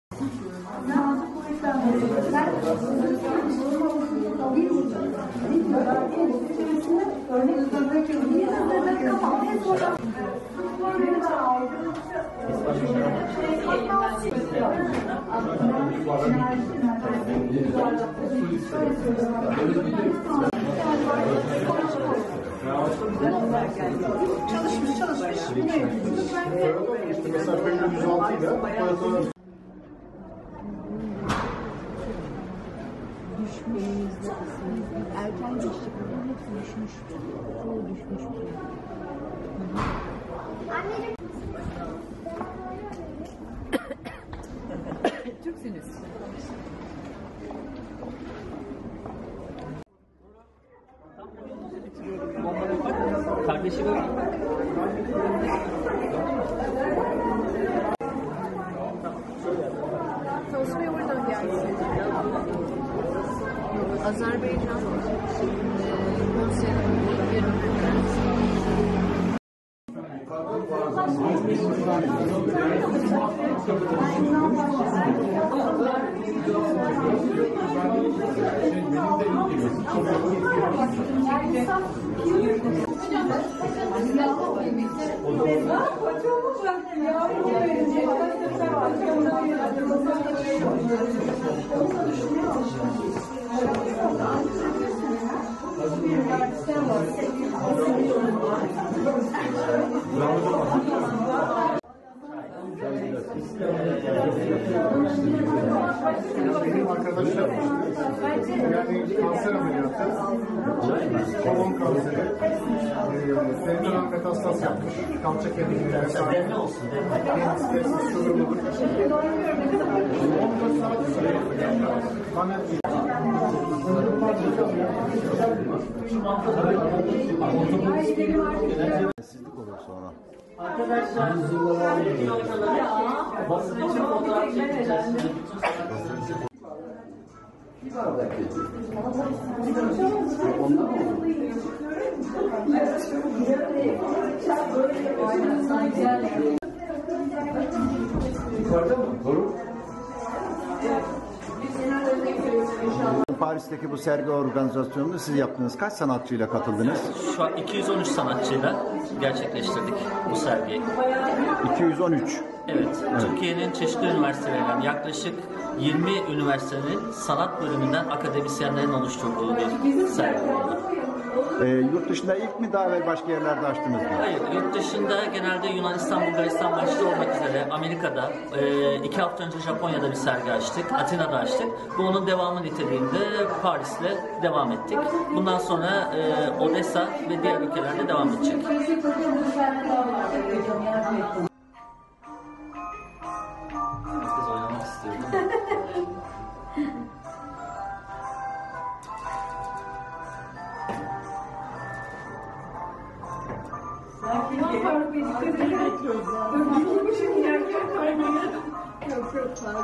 PARİS “DA VİNCİ ART GALERİDE 213 TÜRK AKADEMİSYENİN ESERLERİ SERGİLENDİ
Fransa’nın başkenti Paris’e Türk Akademisyen sanatçılar akını var, Paris’te yerli ve yabancı sanatçıların eserlerini sergilemekle tanınan Faruk Tepe ile Özge Anıl’a ait Da Vinci Art sanat galerisi, her ay birkaç sanatçıya ev sahipliği yapıyor. Sanat Galerisi Da Vinci Art, bu sefer oldukca kalabalık bir sanatçı grubunun eserlerini sergiledi. Resim üzerine özgür çalışmaların ve her renk tonunu barındıran ve resim üzerine, soyut çalışmadan peyzaj çalışmaya kadar bir çok tarz resmin bir arada sergilendiği bir konumundaydı bu sergi.